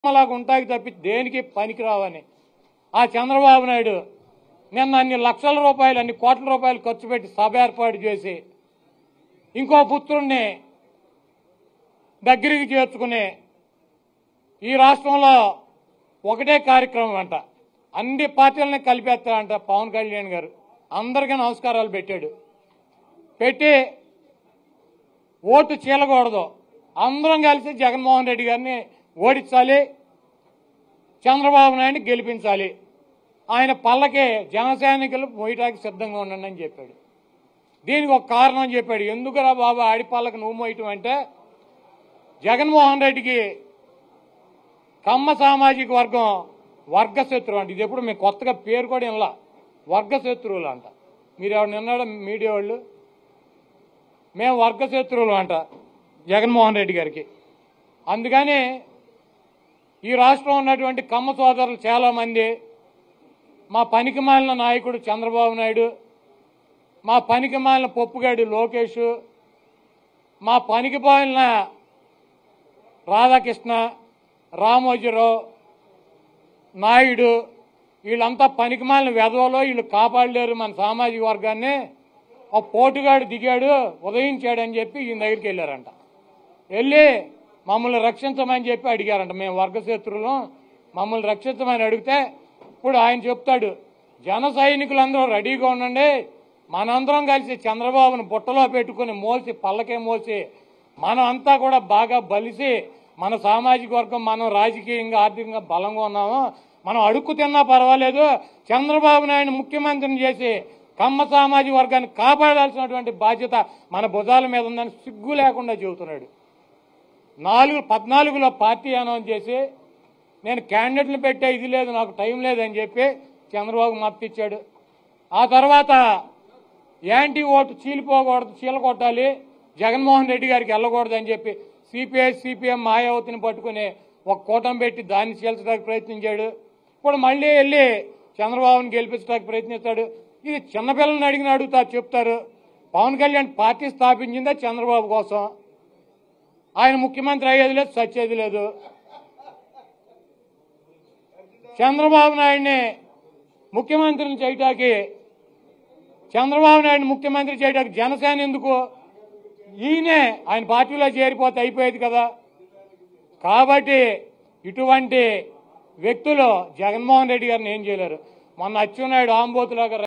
Kodali Nani Satires On Pawan Kalyan | Janasena Avirbhava Maha Sabha | Ntv
LIVE : Minister Kodali Nani Press Meet l NTV Live